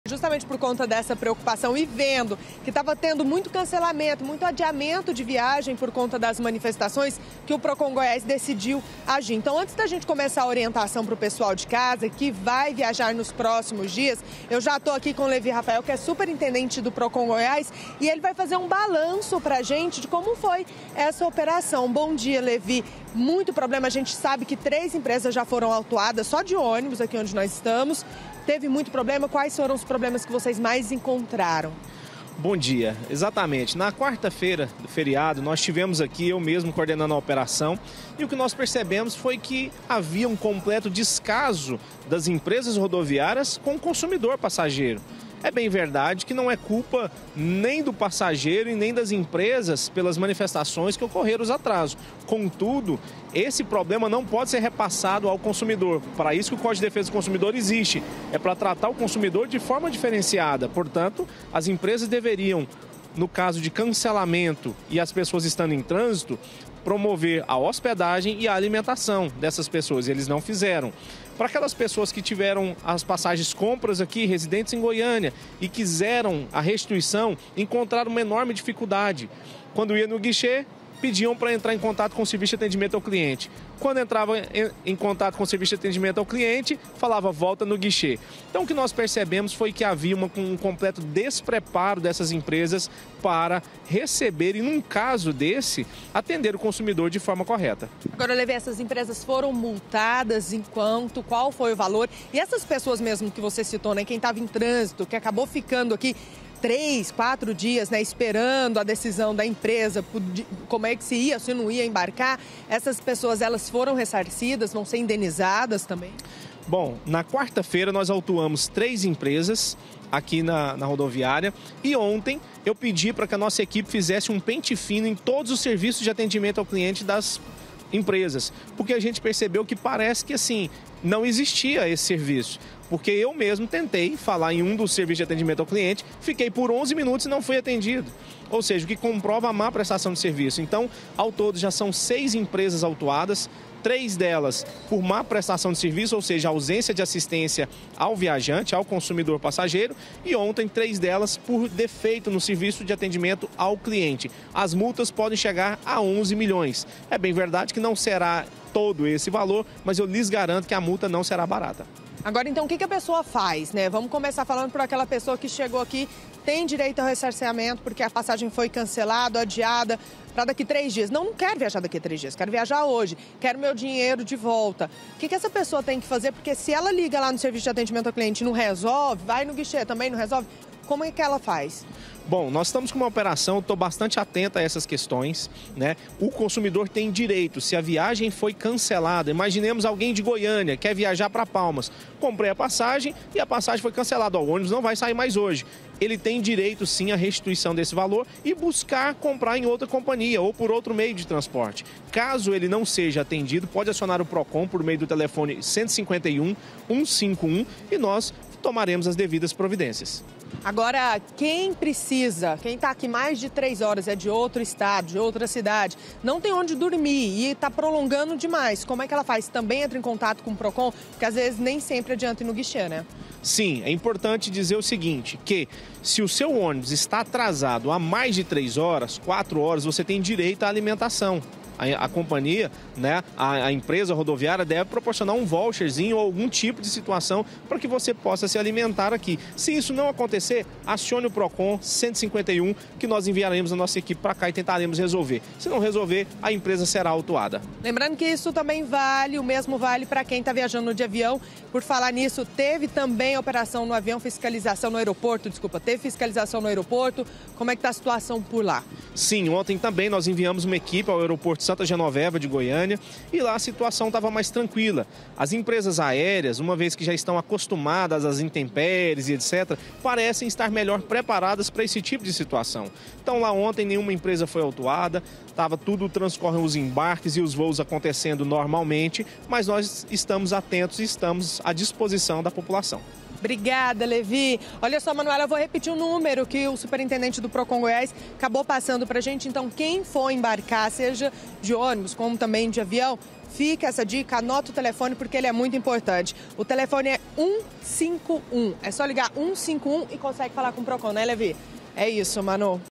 PROCON ORIENTA PASSAGEIROS COM VIAGENS ATRASADAS
PROCON DE GOIÁS AUTUOU TRÊS EMPRESAS DE ÔNIBUS POR MÁ PRESTAÇÃO DE SERVIÇO DURANTE UMA FISCALIZAÇÃO NA RODOVIÁRIA DE GOIÂNIA. MUITOS PASSAGEIROS AINDA ESTÃO COM VIAGENS ATRASADAS POR CAUSA DOS BLOQUEIOS NAS RODOVIAS. VAMOS SABER QUAIS OS DIREITOS DOS CONSUMIDORES.